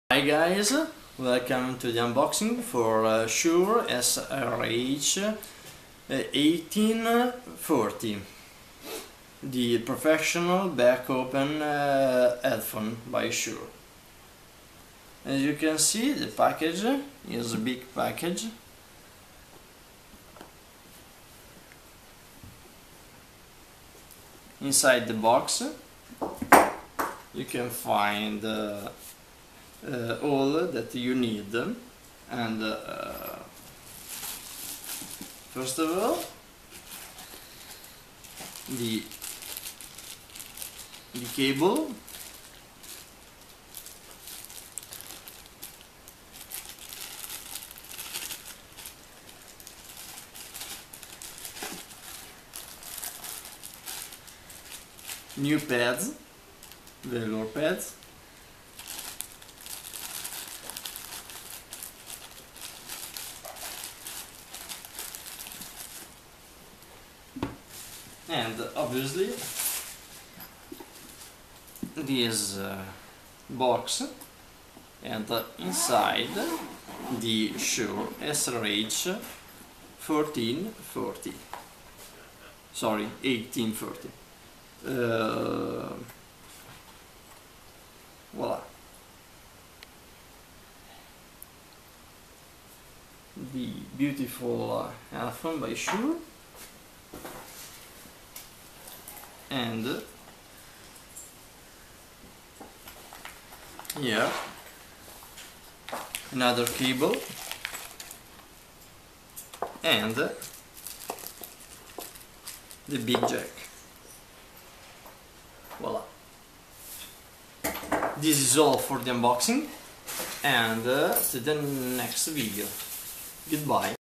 Shure SRH1840: unboxing en
Hi guys;
here the unboxing for Shure SRH1840 the top on open back headphone by Shure.